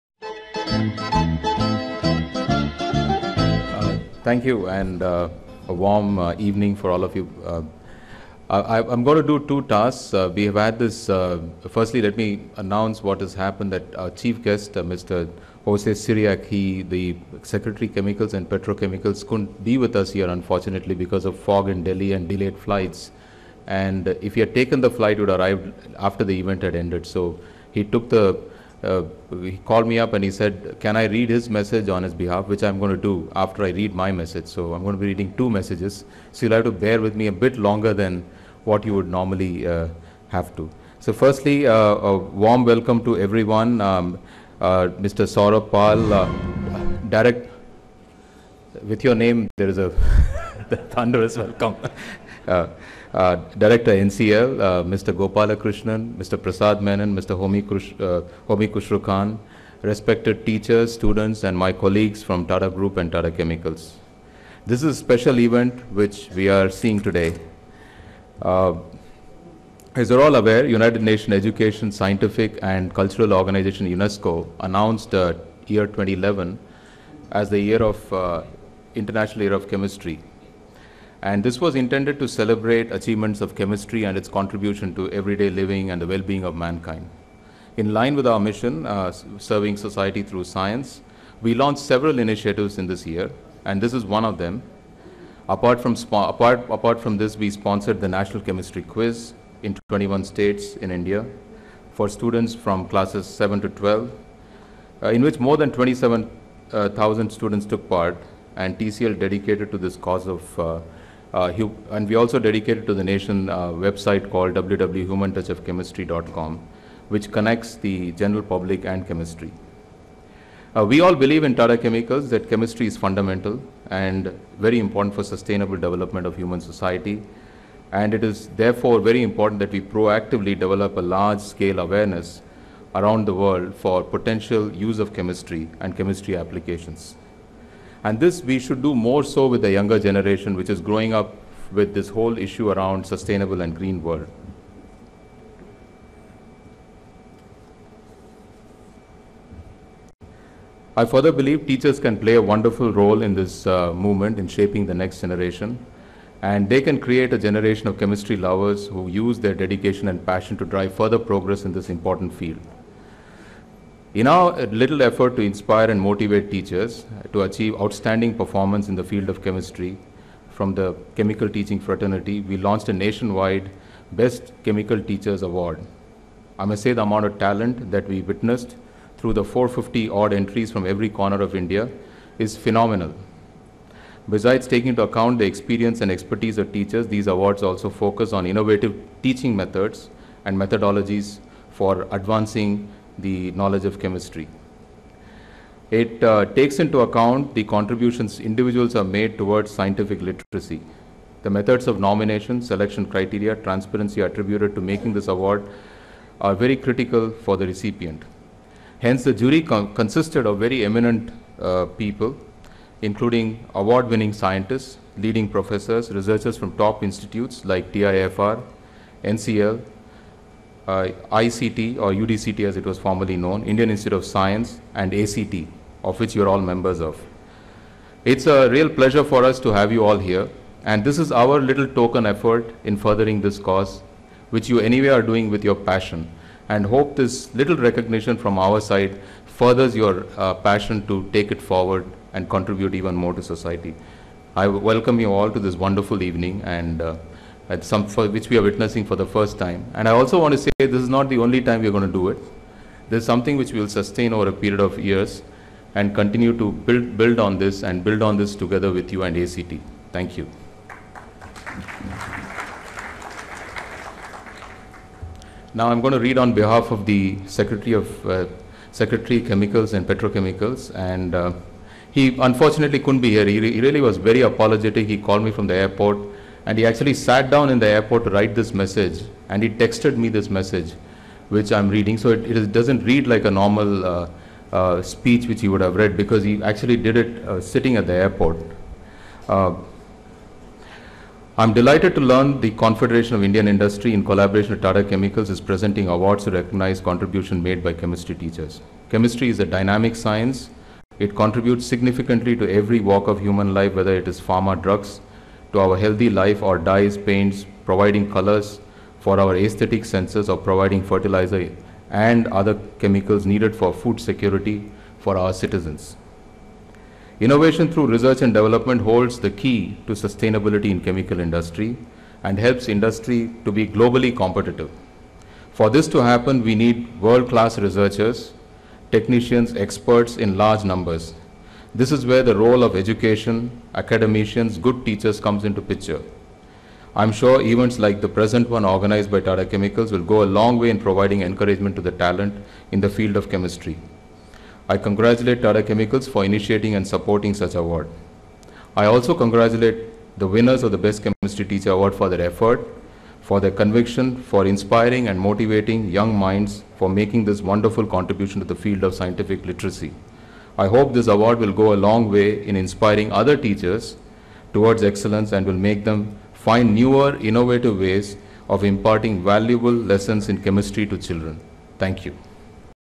R. Mukundan Speech, Best Chemistry Teacher Award 2011
R. Mukundan - The Managing Director of TATA Chemicals Ltd. gives a speech at the Best Chemistry Teacher Awards, 2011.

The Best Chemistry Teacher Award 2011 is presented by Tata Chemicals in collaboration with Association of Chemistry Teachers to honour and recognize excellence in teaching chemistry. This award is open for 10-12th, graduate and post graduate teachers.